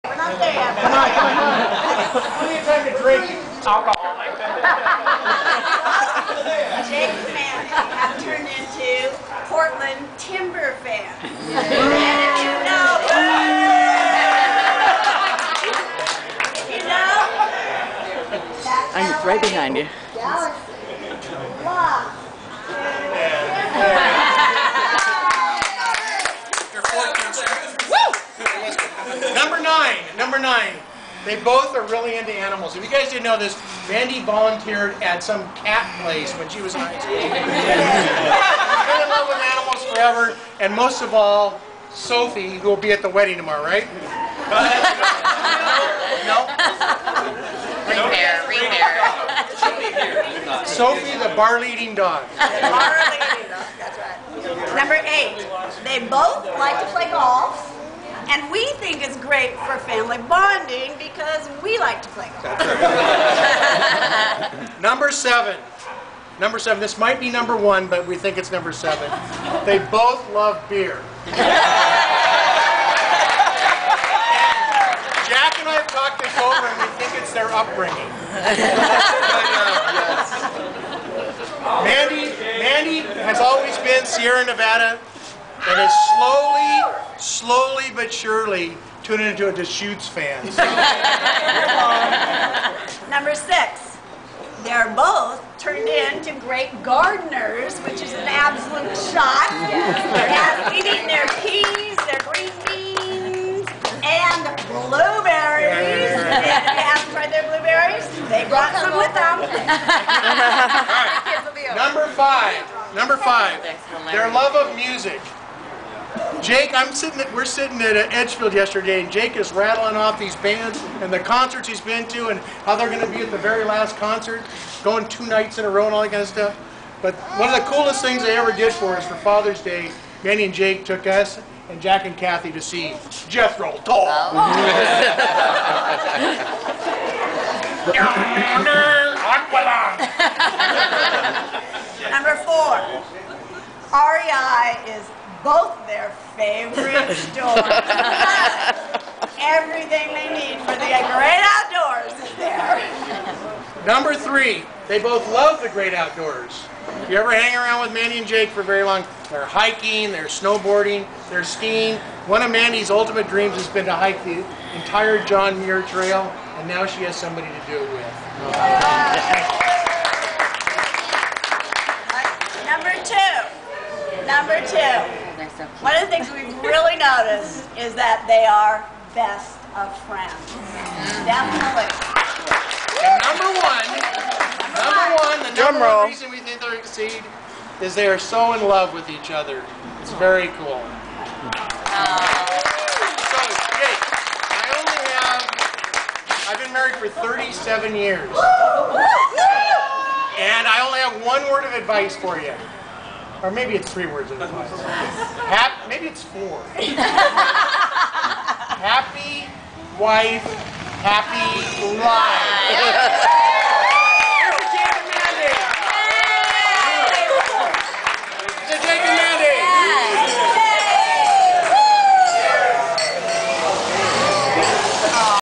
jake and mandy
Betty Ann & Verne top 10 reasons Jake and Mandy are the perfect couple